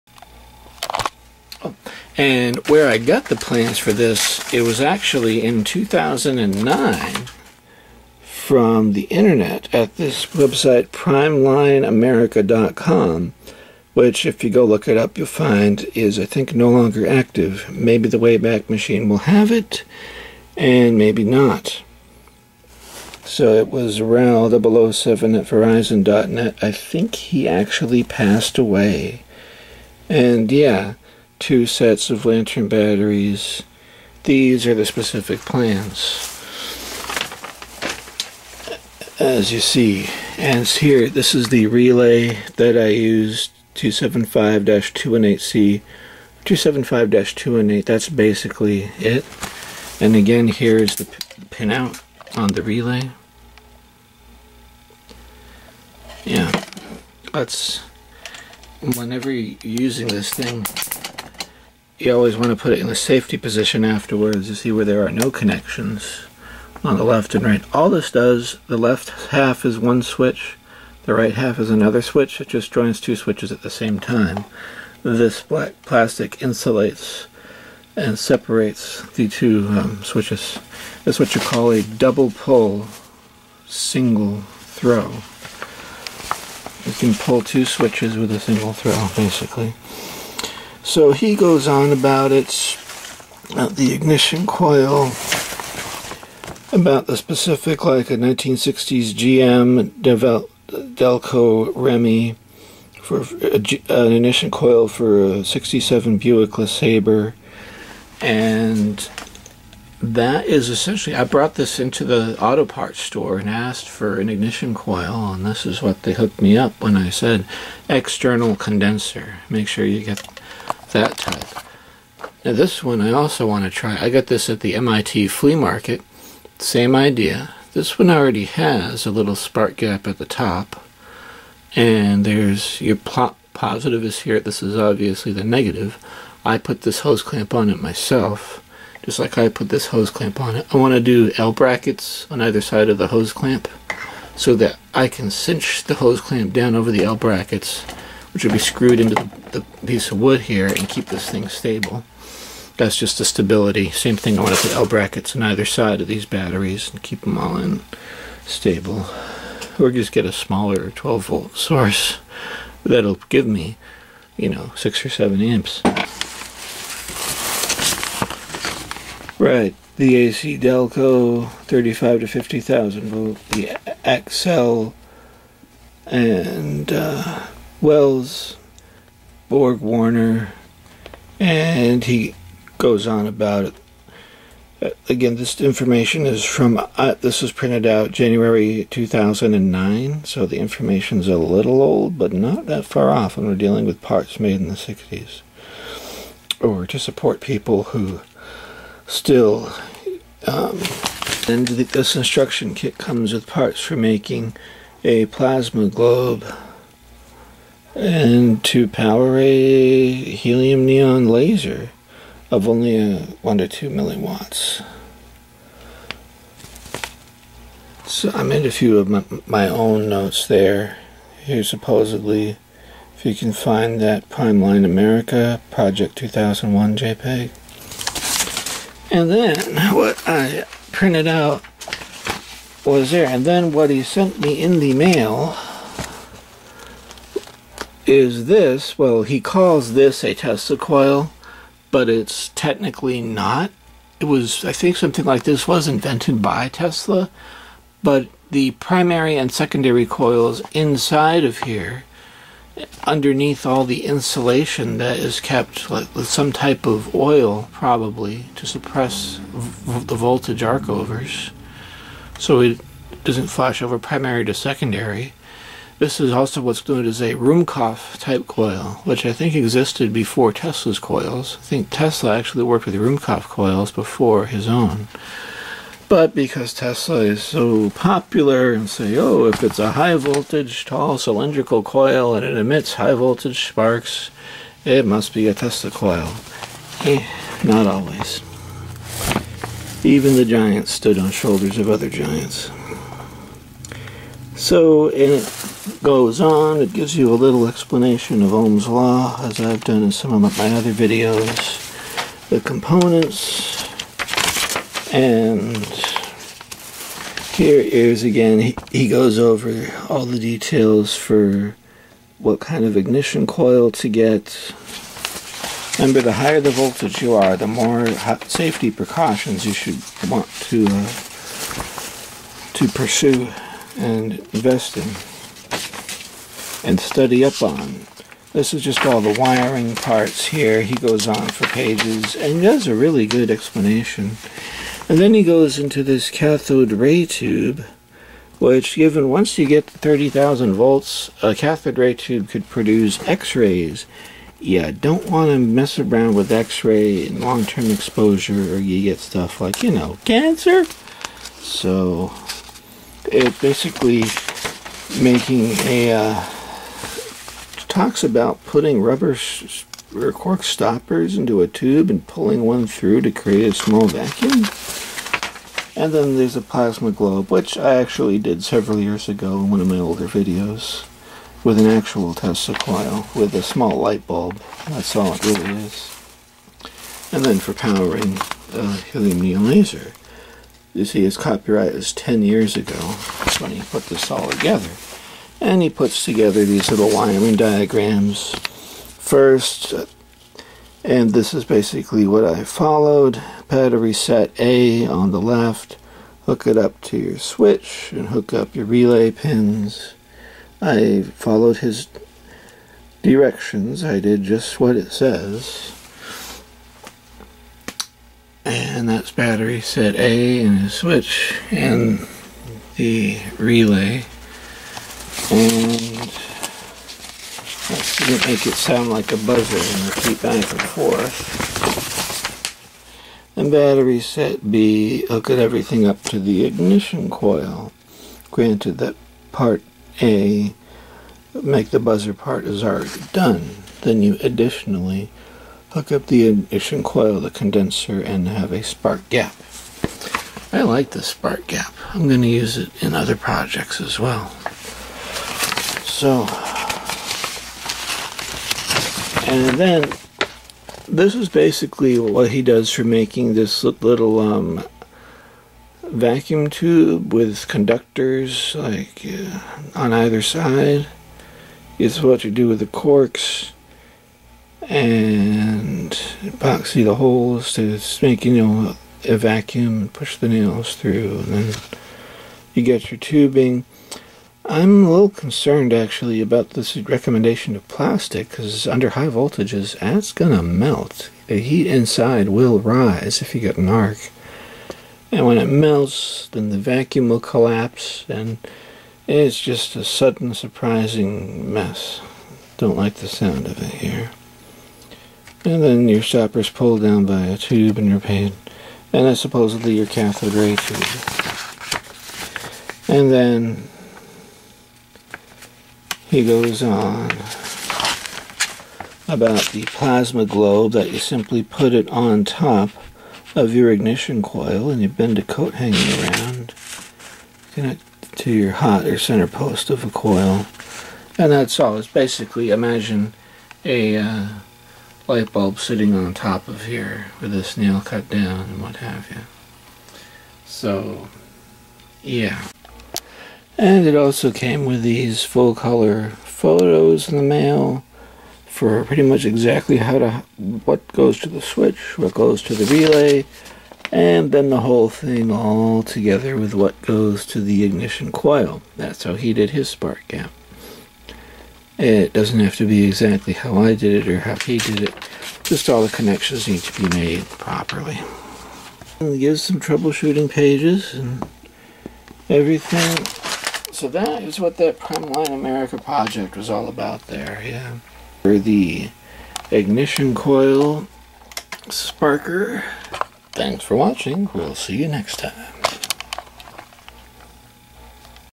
Ignition coil relay driver plans - primeline-america
Here are the plans for the relay driven ignition coil driver vid that I just posted.  I got the plans in early 2009 just before the author/designer of this unfortunately passed away.  He made this video and another one like it back when he was alive and selling the plans on primeline-america.com.
You can see in his video that instead of a relay, he uses a 3 volt motor with a piece of metal on the rotor to make and break the contact, and get the pulsed current to the condenser and ignition coil. It performs the same function as the relay oscillator, as far as this circuit is concerned, but with less voltage.
Here is the archived page of where he sold the plans
Rest in peace, Ray Labonski.